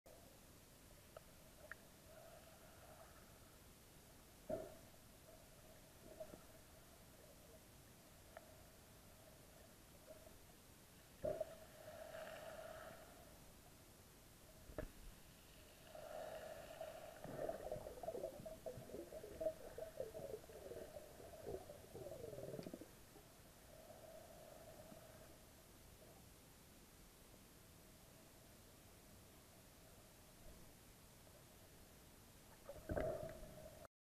octopus shooting ink!
Mediterranean octopus shoots ink at me!